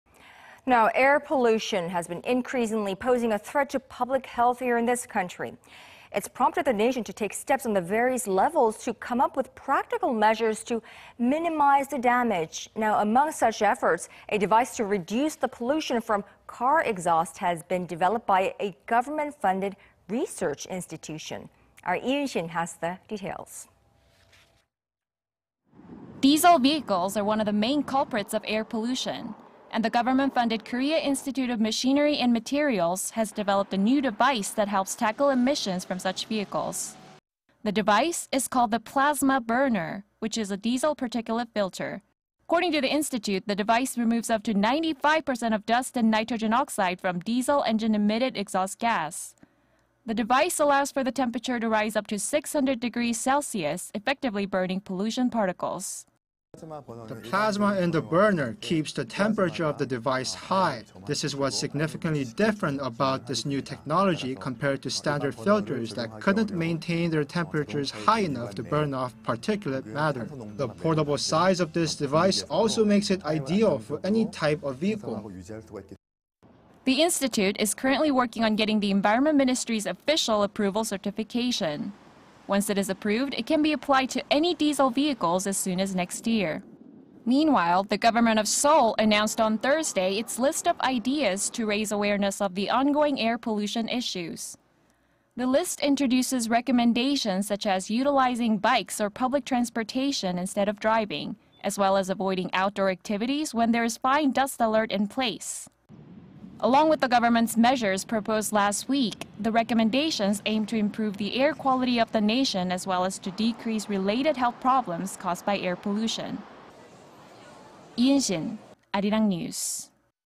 New technology to minimize air pollution developed by government institute
Air pollution has been increasingly posing a threat to public health in Korea.
It's prompted the nation to take steps on the various levels to come up with practical measures to minimize the damage.
Among such efforts, a device to reduce the pollution from car exhaust has been developed by a government-funded research institution.
Lee Un-shin, has the details.

Diesel vehicles are one of the main culprits of air pollution..., and the government-funded Korea Institute of Machinery and Materials has developed a new device that helps tackle emissions from such vehicles.

The device is called the Plasma burner, which is a diesel particulate filter.
According to the institute, the device removes up to 95 percent of dust and nitrogen oxide from diesel engine-emitted exhaust gas.
The device allows for the temperature to rise up to 600 degrees Celsius, effectively burning pollution particles.

"The plasma in the burner, keeps the temperature of the device high.
This is what's significantly different about this new technology compared to standard filters that couldn't effectively maintain their temperatures high enough to burn off particulate matter.
The portable size of this device also makes it ideal for any type of vehicle."

The institute is currently working on getting the environment ministry's official approval certification.
Once it is approved, it can be applied to any diesel vehicles as soon as next year.

Meanwhile, the government of Seoul announced on Thursday its list of ideas to raise awareness of the on-going air pollution issue.
The list introduces recommendations such as utilizing bikes or public transportation instead of driving, as well as avoiding outdoor activities when there's a fine dust alert in place.

Along with the government's measures proposed last week, the recommendations aim to improve the air quality of the nation as well as to decrease related health problems caused by air pollution.

Lee Un-shin, Arirang News.